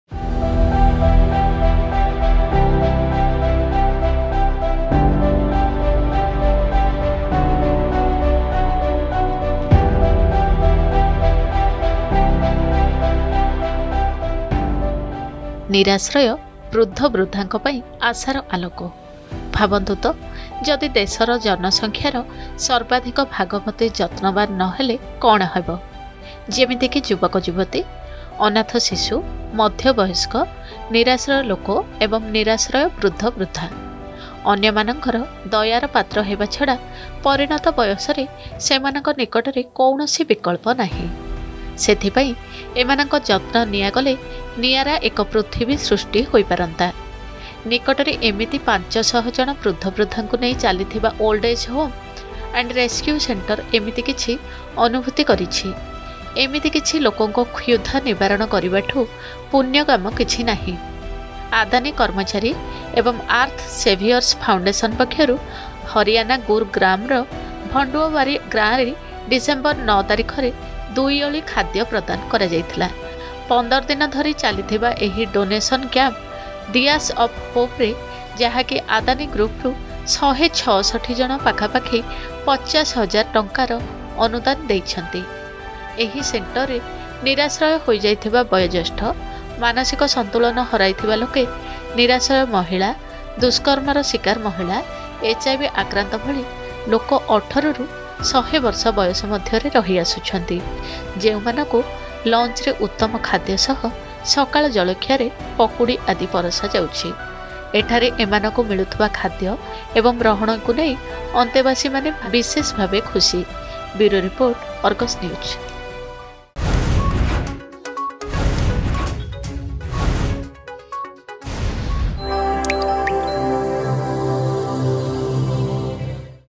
ନିରାଶ୍ରୟ , ବୃଦ୍ଧବୃଦ୍ଧାଙ୍କ ପାଇଁ ଆଶାର ଆଲୋକ
ଆଦାନୀ କର୍ମଚାରୀ ଏବଂ ଆର୍ଥ ସେଭିଅର୍ସ ଫାଉଣ୍ଡେସନ ପକ୍ଷରୁ ସହାୟତା। ହରିୟାଣା ଗୁରୁଗ୍ରାମର ଭଣ୍ଡଓୱାରି ଗାଁରେ ଡିସେମ୍ବର ୯ରେ ଅସହାୟଙ୍କୁ ଦୁଇଓଳି ଖାଦ୍ୟ ପ୍ରଦାନ କରାଯାଇଥିଲା।